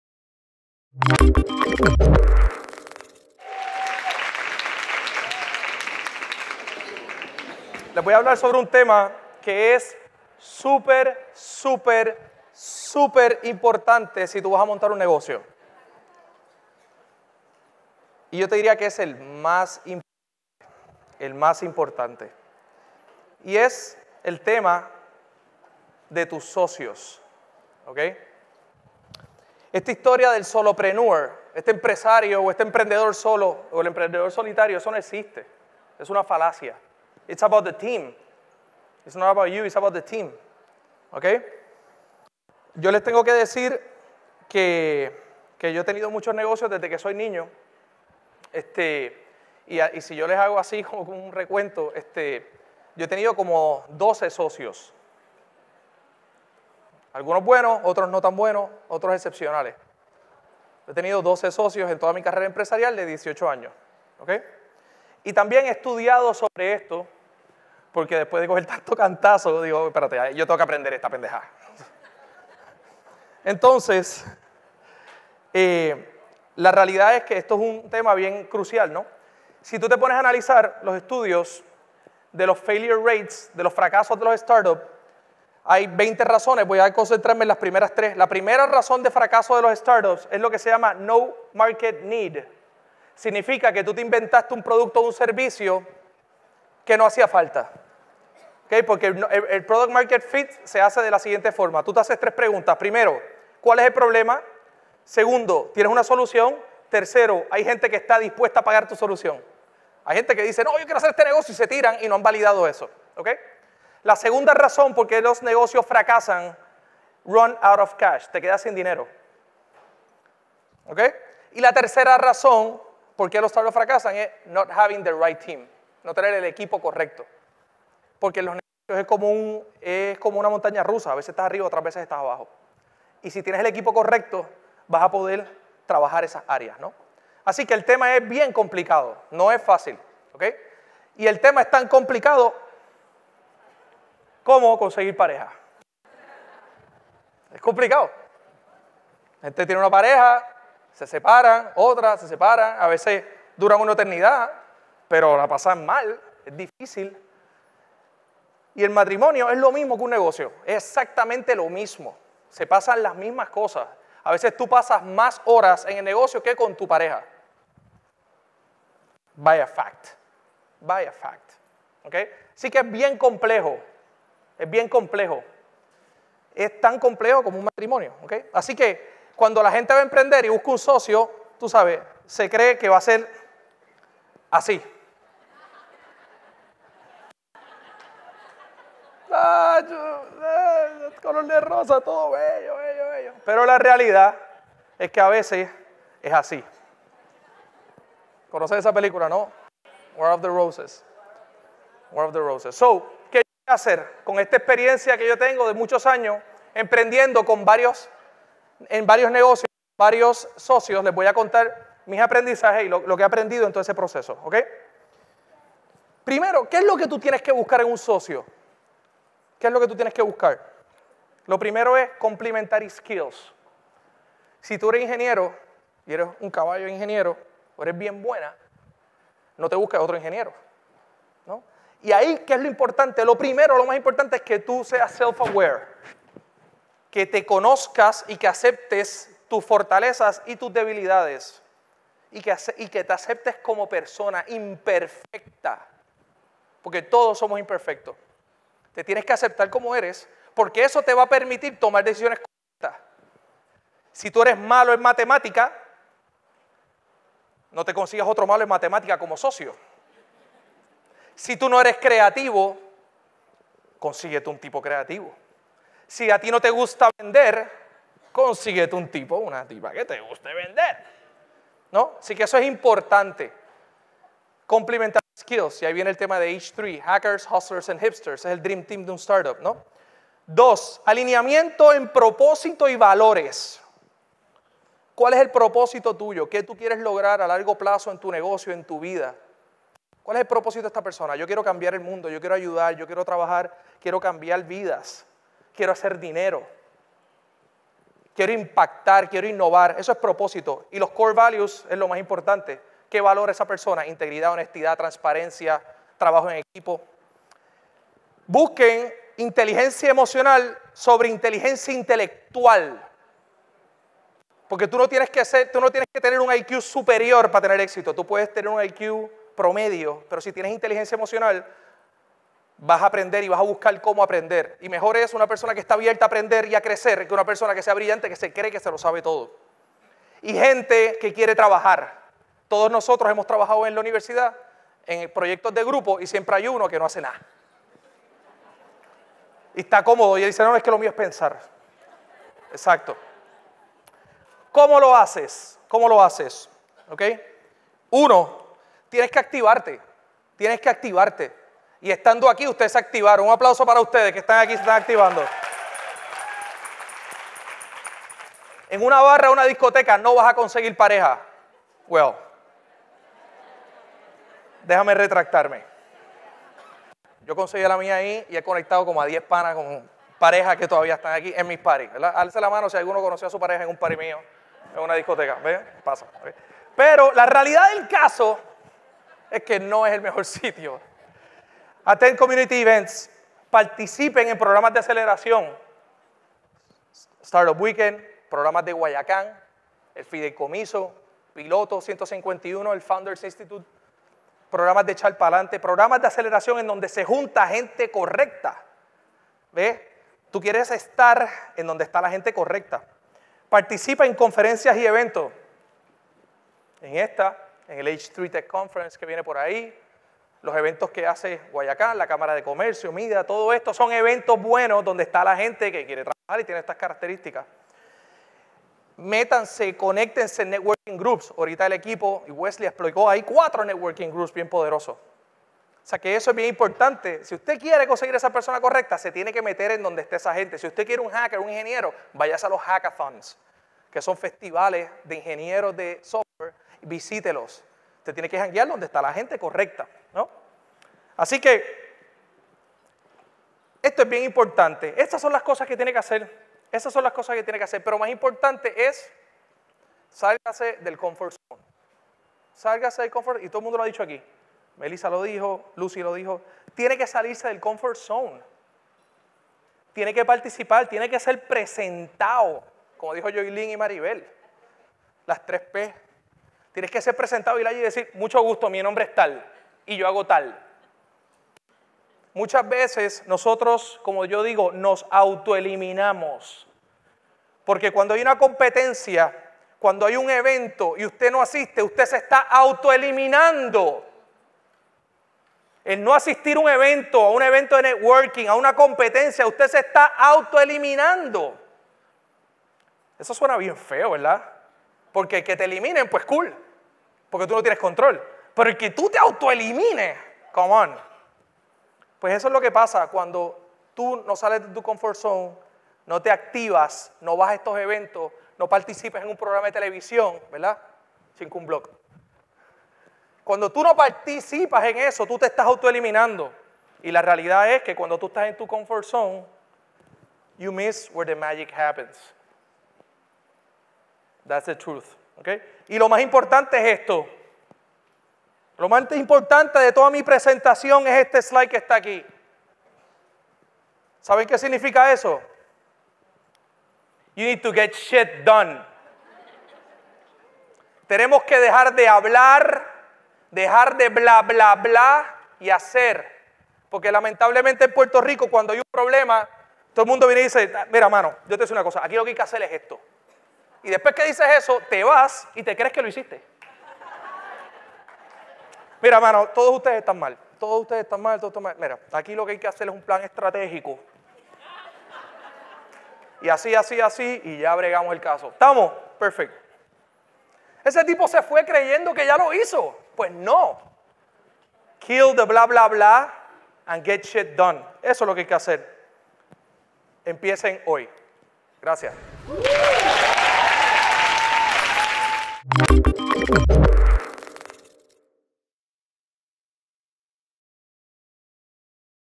Animus Summit 2015 - Carlos Cobian
During his address at the 2015 Animus Summit, event co-founder Carlos Cobian discusses the importance of putting together the right team and partnerships when building your business.

Carlos R. Cobián is an accomplished serial entrepreneur recognized for developing successful businesses in Latin America.  In 1995, when he was 21 years old and performing as a club Dj, he co-founded Empire Entertainment, Puerto Rico’s first youth and experiential marketing agency. Between 1995 and 2006 this company became the leading experiential marketing agency in the Island and managed leading brands such as Coors Light, Bacardi, Toyota, Pepsi and Hilton Hotels among many others. Empire disrupted the traditional advertising agency business by promoting new performance based business models, propelling new ways of marketing to youth and promoting entrepreneurship among Puerto Rico’s youth, resulting in the creation of a new wave of creative promotions and production shops.

After a career of almost 20 years he has owned restaurants, bars, discotheques, a marketing agency and a mobile technology company called Wireless Idea. This company was a pioneer in the use of the mobile devices as a marketing tool for brands. It also helped content providers  monetize their content via  mobile devices across 17 countries in Latin America.  He also founded Cobian Media, a company that produces special events and digital marketing to companies who want to expand and grow.  He is the founder of the H3 Tech Conference and serves as board member for The San Juan Tech Meet Up and Centro para Emprendedores.  He is also a mentor of the Founders Institute of Silicon Valley.